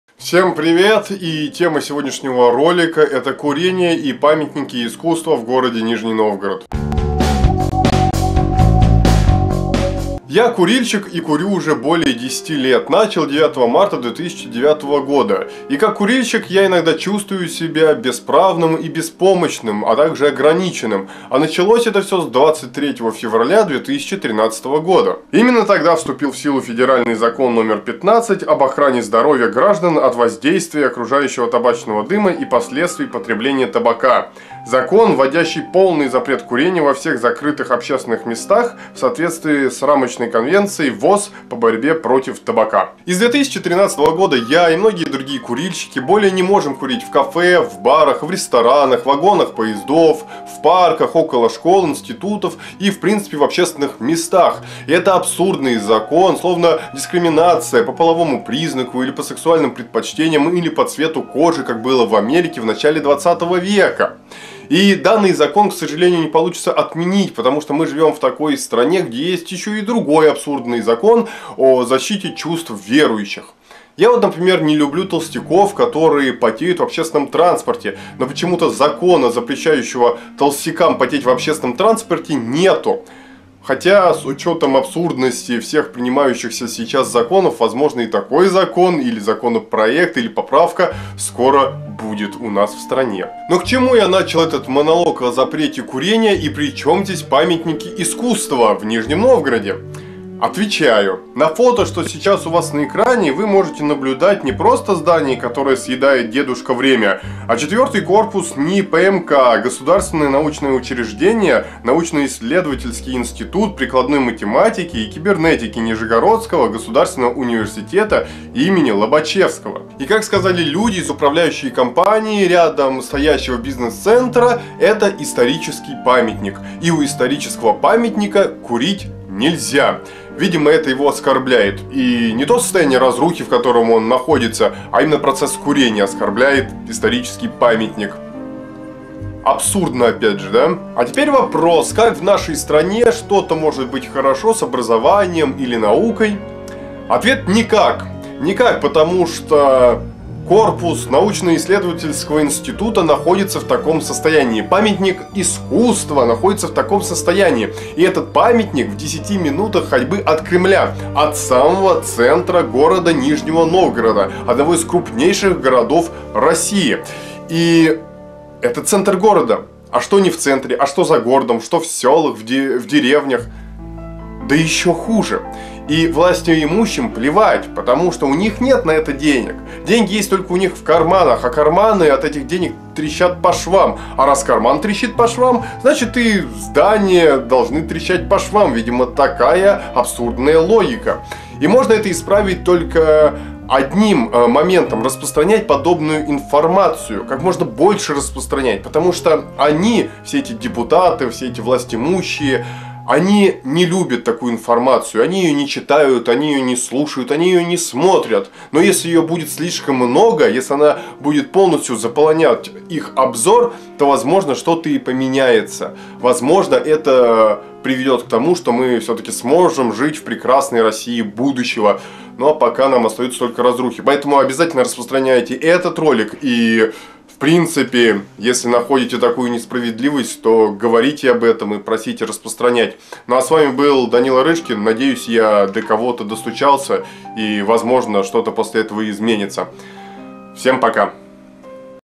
Разрушение памятников и оскорбление курильщиков в Нижнем Новгороде
Сегодня поговорю о законе о запрете курения и о разрушениях памятников в городе Нижний Новгород.